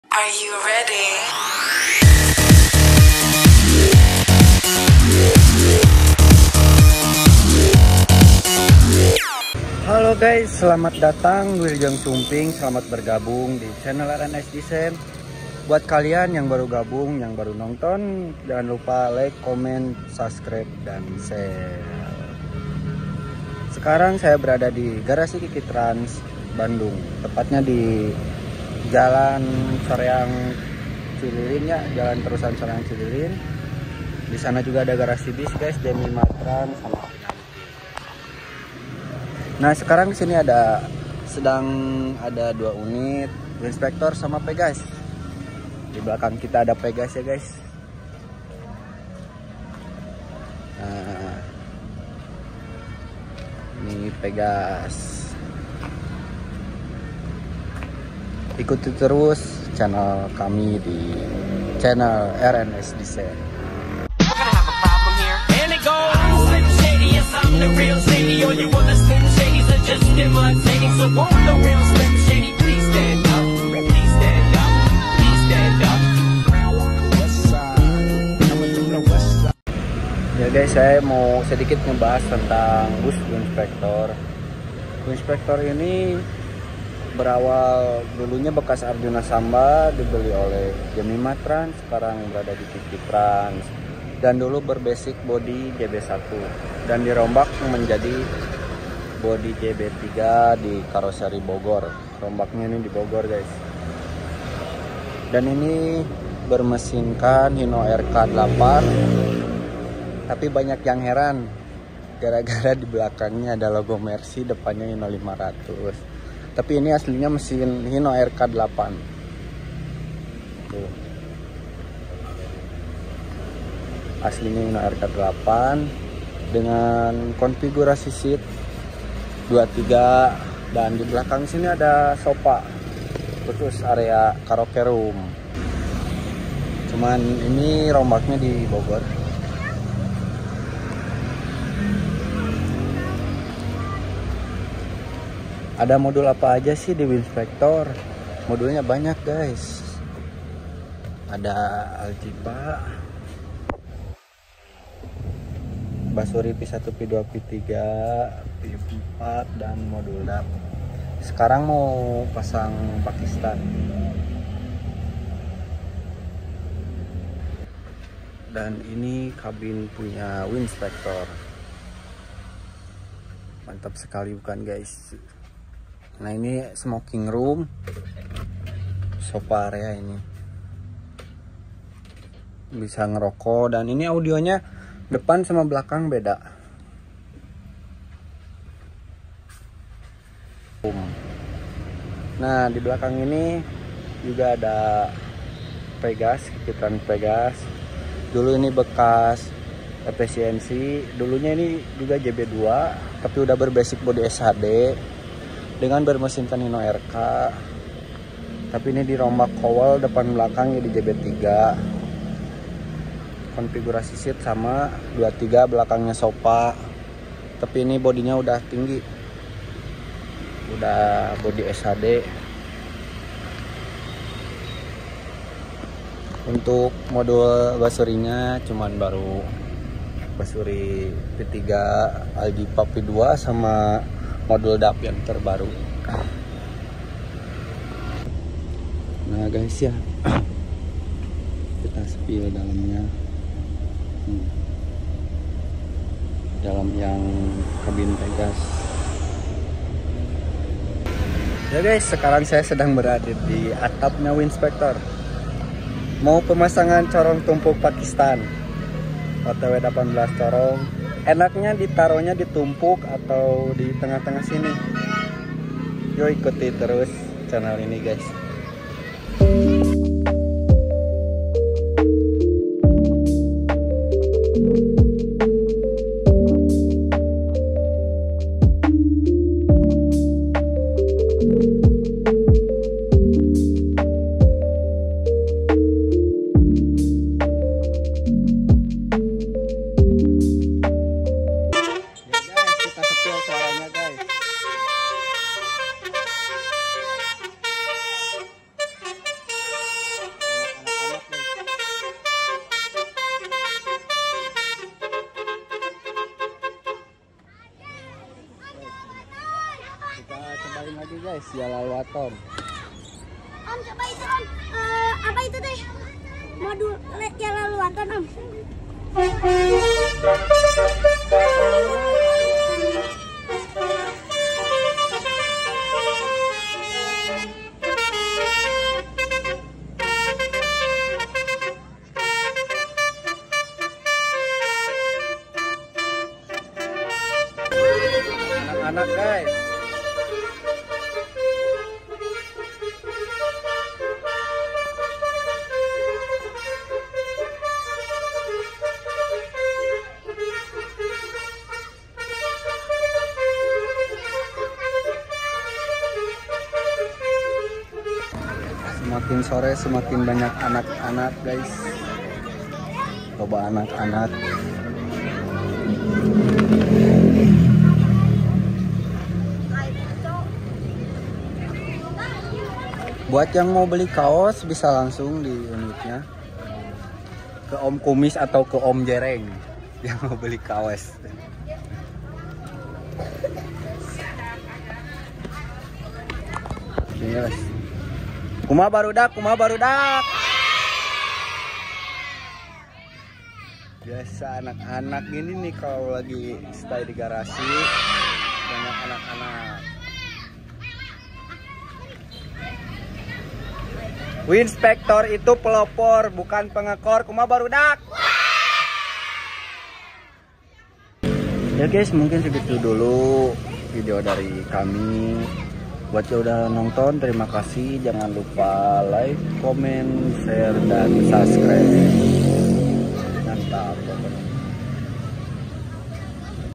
Garasi qq trans winspector Dan Vegas’s
Vlog gabut di Garasi sekali Gus pemasangan corong pakistan winspector mohon Maaf gaise video Nya Ngga rapih Karena saya masih belajar 🙏🏻